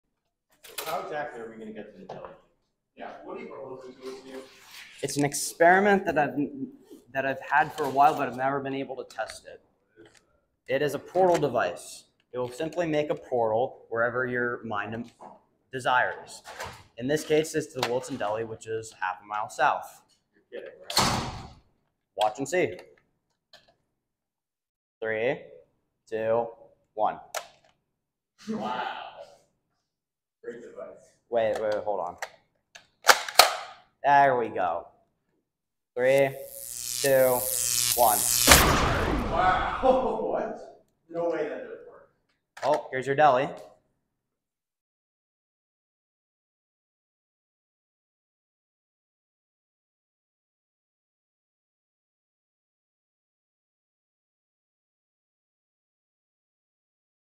Portal gun short film (Trailer)
This is a trailer for my short film, this file alone is 6 gb so idk how big the final project will be but hope you enjoy!